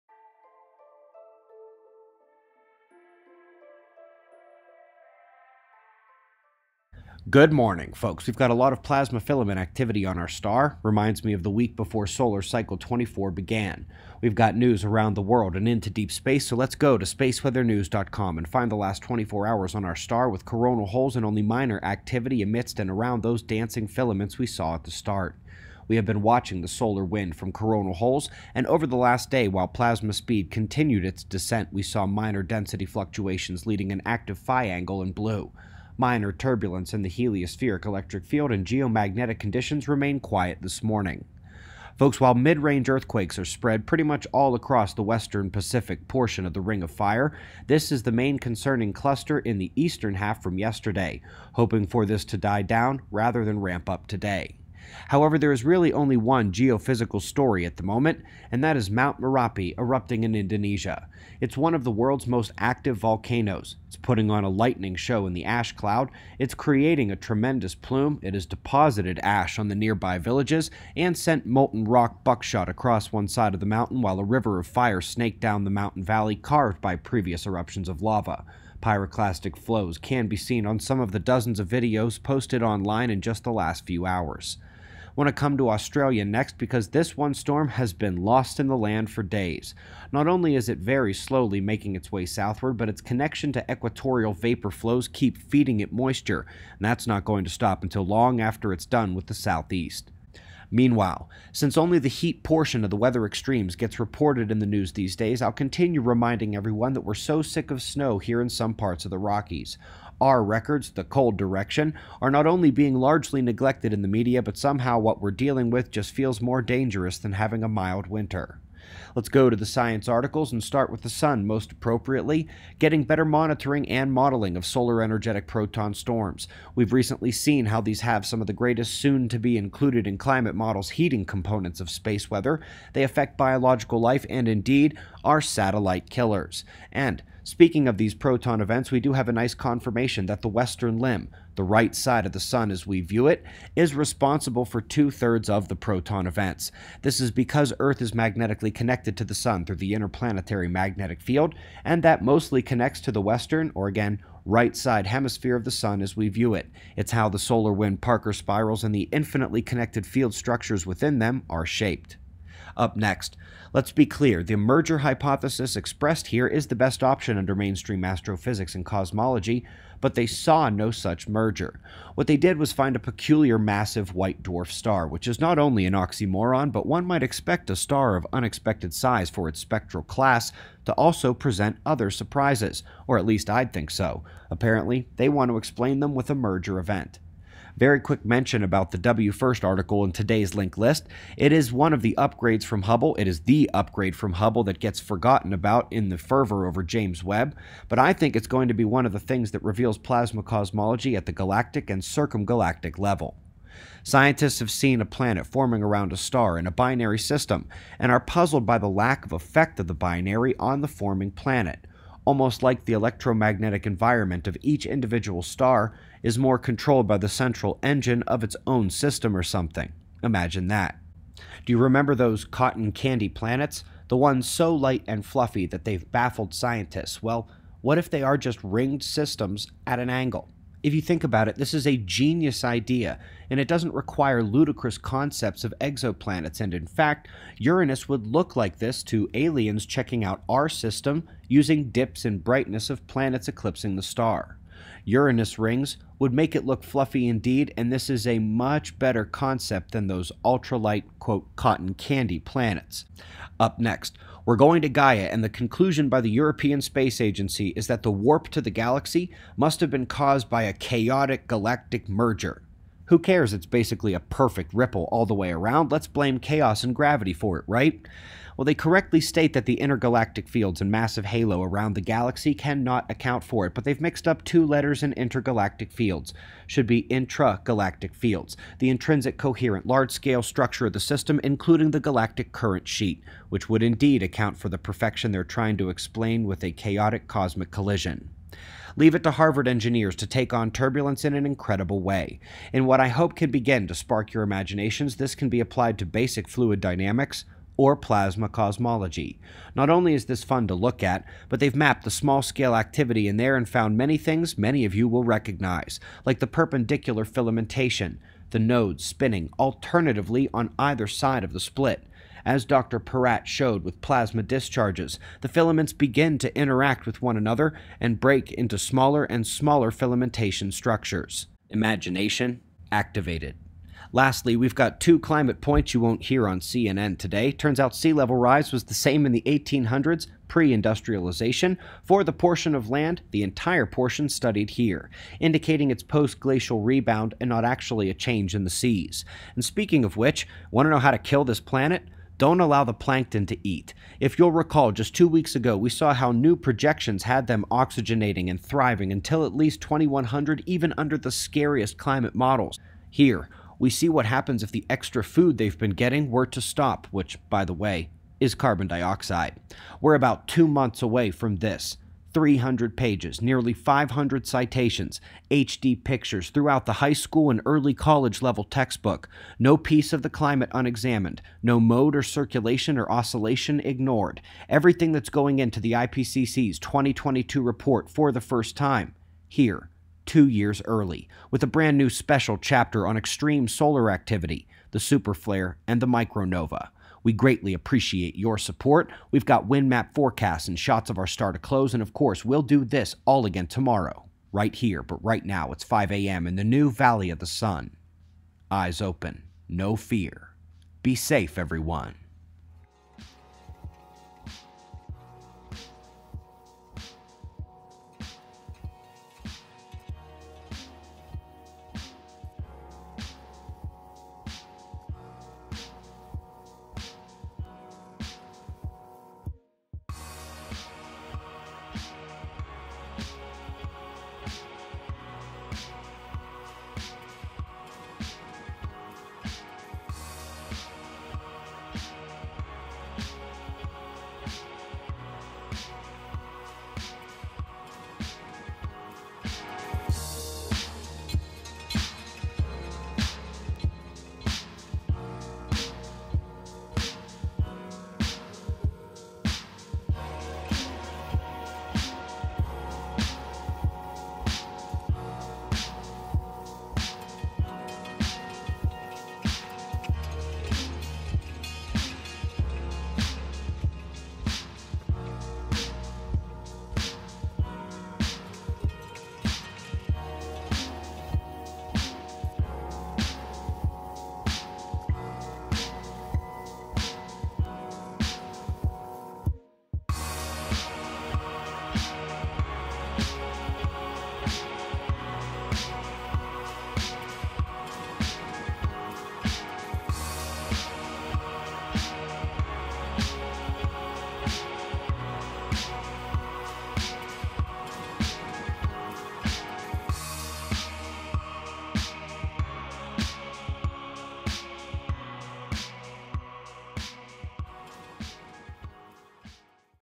Volcano, Oceans, Galactic Warp, Familiar Turbulence | S0 News Mar.3.2020
~2 Months Until Our 3rd Edition of the Textbook s Available